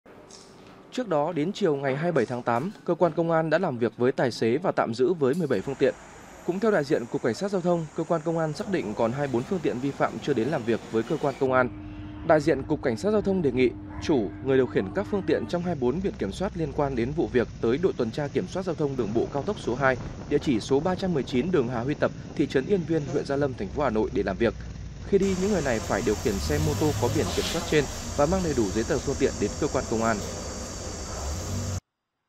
Đoàn xe phân khối lớn đi vào cao tốc: CSGT đề nghị 24 chủ xe đến cơ quan công an | Tin nóng
Liên quan đến đoàn xe phân khối lớn đi vào cao tốc Bắc Giang - Lạng Sơn, đại diện Cục CSGT cho biết, đơn vị đã tạm giữ 17 phương tiện, hiện còn 24 chủ xe chưa đến làm việc với cơ quan công an.